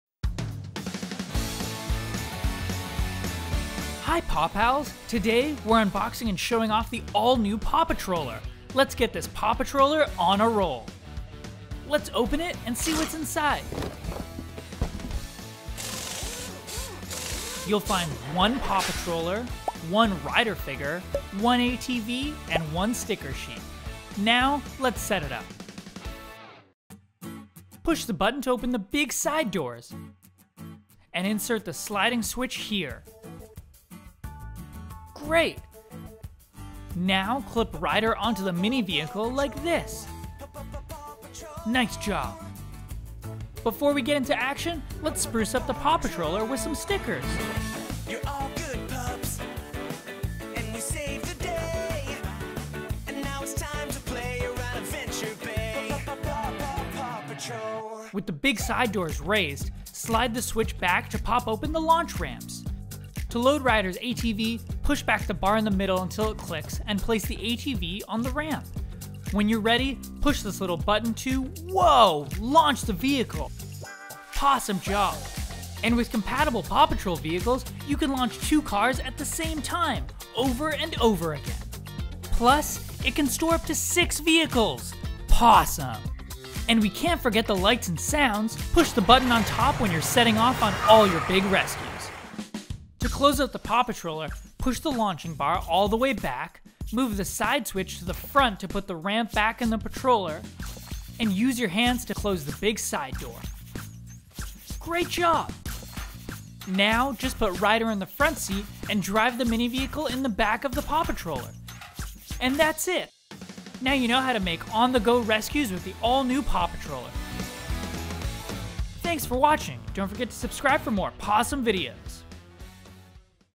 PAW Patrol - All-New PAW Patroller – How to Play & Save the Day!
Learn how to make big rescues with the all-new, big PAW Patroller! With room for 6 Pups and dual vehicle launchers, you can save the day over and over! PAW Patrol is on a roll!